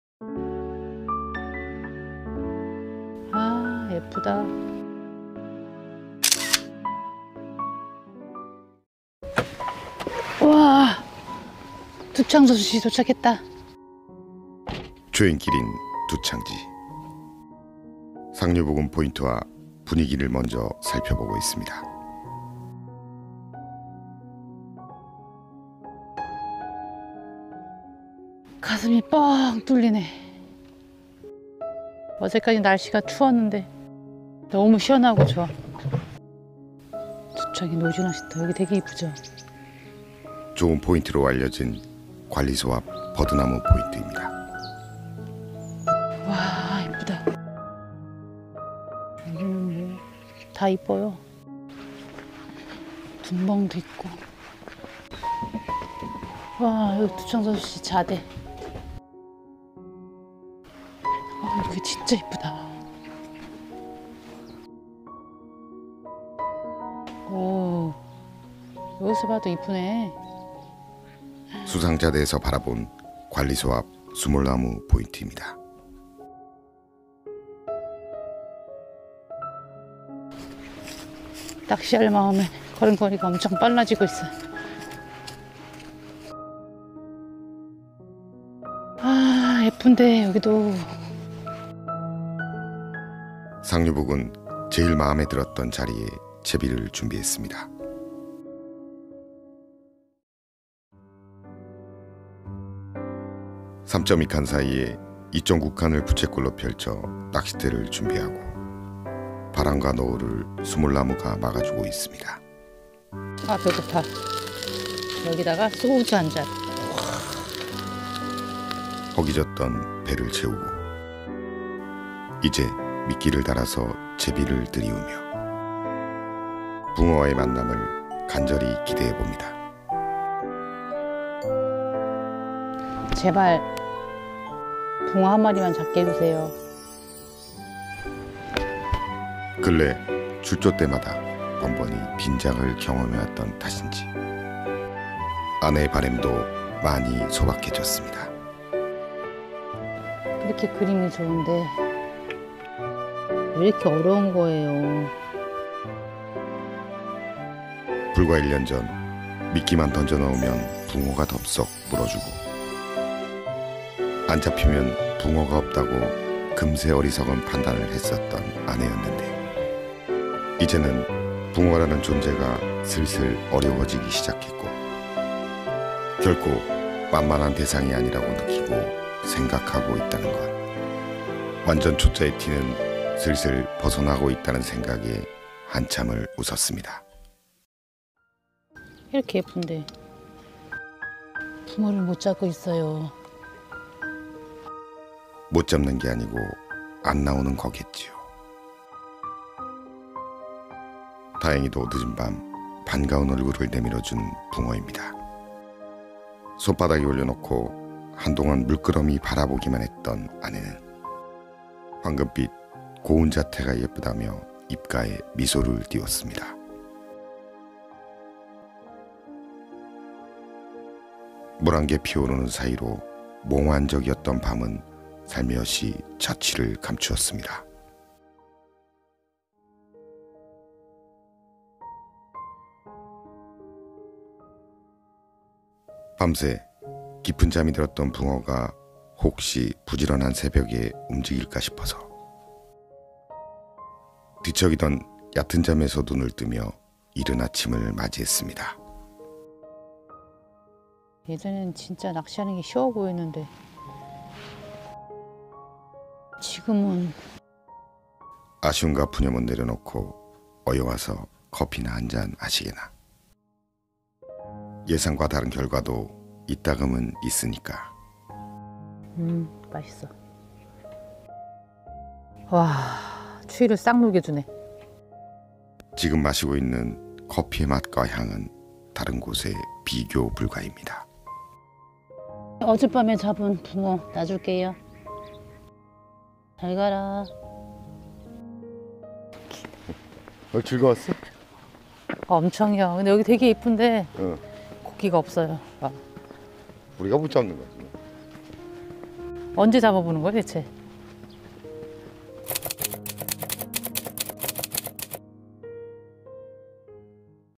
붕어낚시 vlog. 짙어가는 브라운 컬러와 막바지 푸름의 조화가 아름다웠던 두창지. feat. 두창저수지.
경기도 용인 두창저수지에서 하룻밤 편하게 힐링하고 온 붕어낚시 조행기입니다.

▪︎4K로 시청이 가능한 영상입니다.